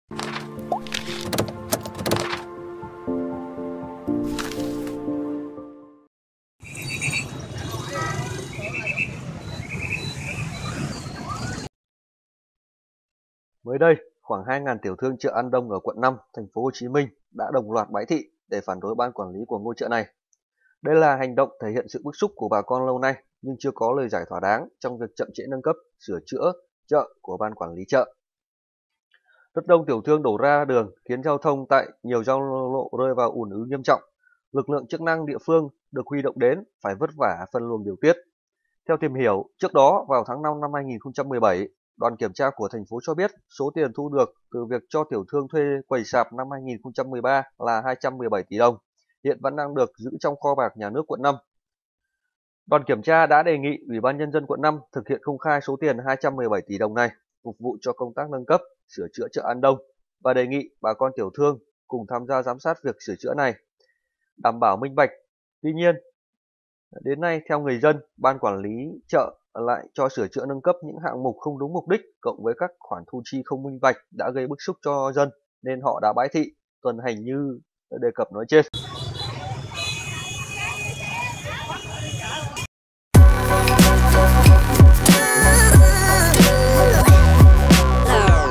Không ngờ hàng ngàn tiểu thương ở Sài Gòn lại dám xuống đường phản ứng như thế này - Tin tức mới
Kênh thông tin VietNam News là kênh thông tin hàng đầu, cập nhật liên tục các vấn đề xã hội, giải trí, ẩm thực...